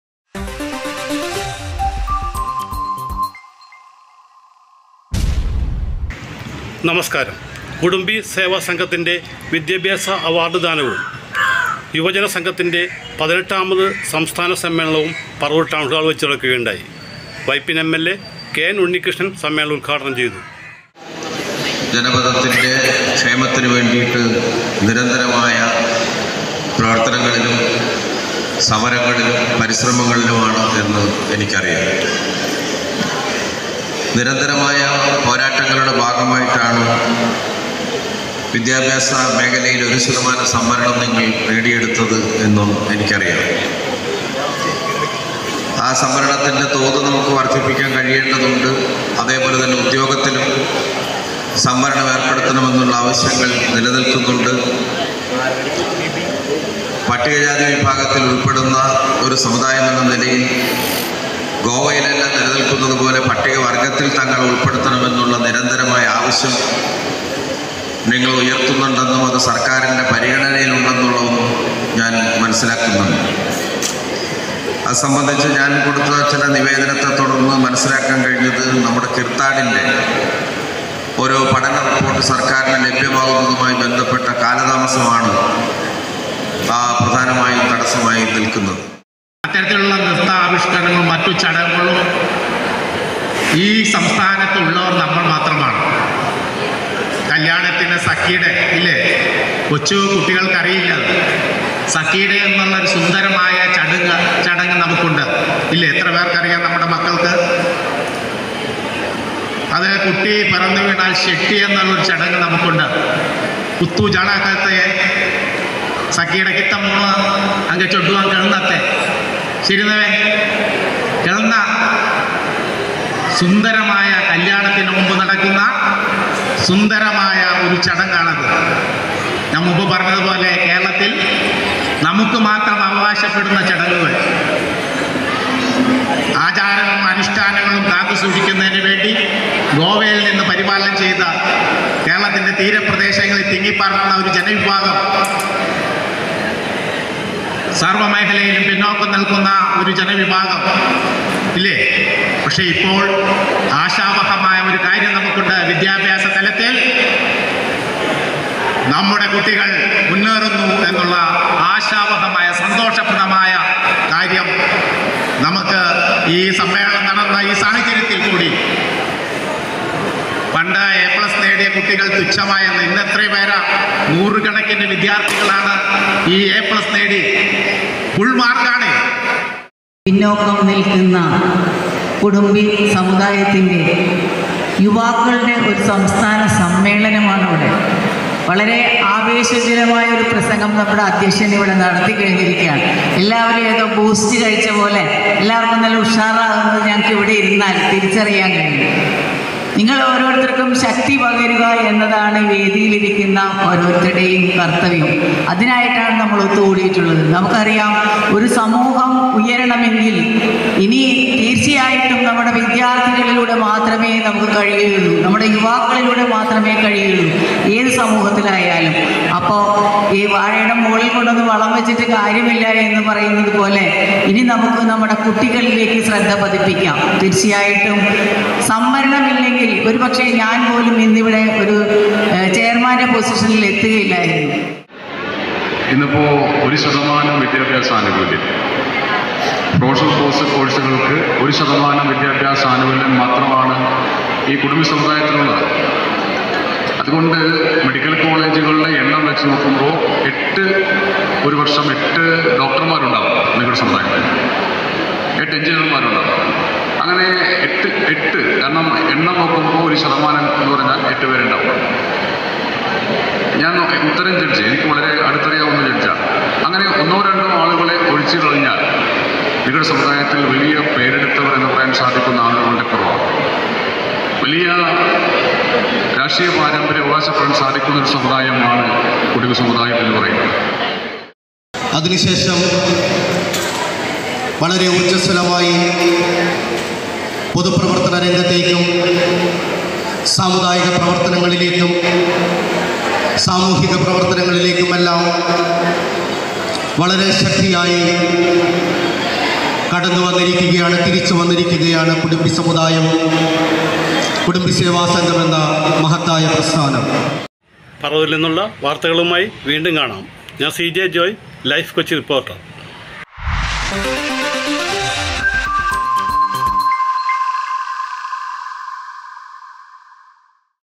നോർത്ത് പറവൂർ : കുഡുംബി സേവാ സംഘം അവാർഡ്ദാനവും യുവജന സമ്മേളനവും നടന്നു.
നോർത്ത് പറവൂർ : കേരള കുഡുംബി സേവാ സംഘം വിദ്യാഭ്യാസ അവാർഡ് വിതരണവും യുവജന സംഘം 18-ാം സംസ്ഥാന സമ്മേളനവും ദീപശിഖ , പതാക പ്രയാണ റാലിക്കു ശേഷം പറവൂർ ടൗൺ ഹാളിൽ വച്ച് നടന്നു. വൈപ്പിൻ എം.എൽ.എ. കെ.എൻ ഉണ്ണികൃഷ്ണൻ ചടങ്ങ് ഉദ്ഘാടനം ചെയ്തു. കുഡുംബി സംഘം സംസ്ഥാന പ്രസിഡന്റ് കെ.സി സുരേഷ് അദ്ധ്യക്ഷനായി. നഗരസഭ ചെയർപേഴ്സൻ വി.എ. പ്രഭാവതി, ബി.ജെ.പി ജില്ലാ പ്രസിഡന്റ് കെ.എസ് ഷൈജു, കുഡുംബി സംഘം സംസ്ഥാന ജനറൽ സെക്രട്ടറി ടി എസ് ശരത്കുമാർ , സംഘം യുവജന നേതാക്കളായ എം.ഡി. അരുൺ, പി.എ. അനൂപ്, എം.പി. പ്രിയേഷ്, ഇ.എൽ. അനിൽകുമാർ, സേവാ സംഘം നേതാക്കളായ ജി.രാജൻ, ബിന്ദു സുബ്രഹ്മണ്യൻ, പാർത്ഥിവ് എം. കുമാർ ,ടി.ആർ രാജേഷ്, രമ്യ ദിലീപ്, എം.ജി മനു, ഉഷ ഗണേശൻ, വി.എൽ രാധാകൃഷ്ണൻ , സി.പി നിധീഷ് , ടി.എസ് സുമേഷ് എന്നിവർ സംസാരിച്ചു. വാർത്തയുമായി ലൈഫ്കൊച്ചി റിപ്പോർട്ടർ സി ജെ ജോയ്.